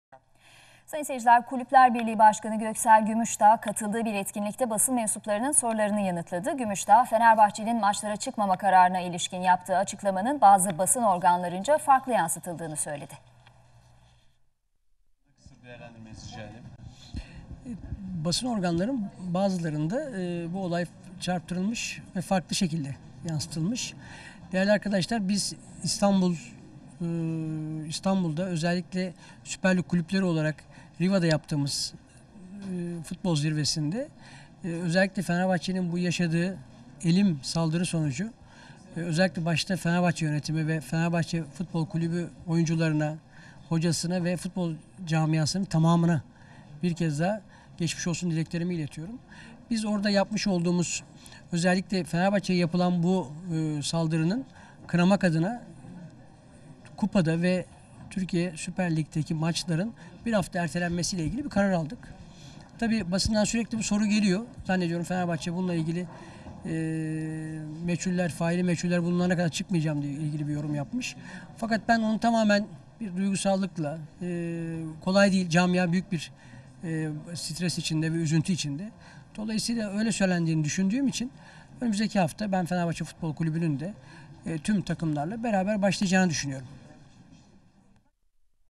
Burcu Eken - Göksel Gümüşdağ
Kulüpler Birliği ve İstanbul Başakşehir FK Başkanı Göksel Gümüşdağ, Çaykur Rizespor deplasmanından dönerken silahlı saldırıya uğrayan Fenerbahçe'nin "Faili meçhuller ortaya çıkana kadar lig'e çıkmam" sözleri üzerine yaptığı açıklamanın yanlış anlaşıldığını açıkladı..